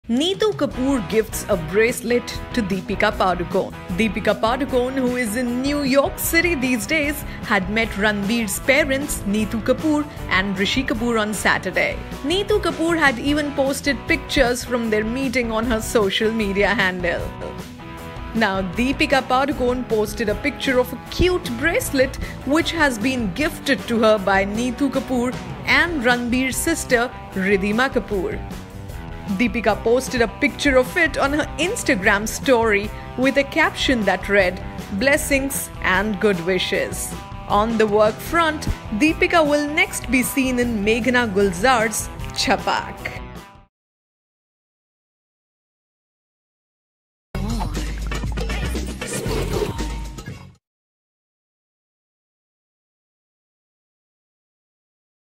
Neetu Kapoor Gifts A Stunning Bracelet To Deepika Padukone!
Ranbir Kapoor's mother and sister Neetu and Riddhima gifted Deepika Padukone a stunning bracelet. Deepika shared a picture on her Instagram account.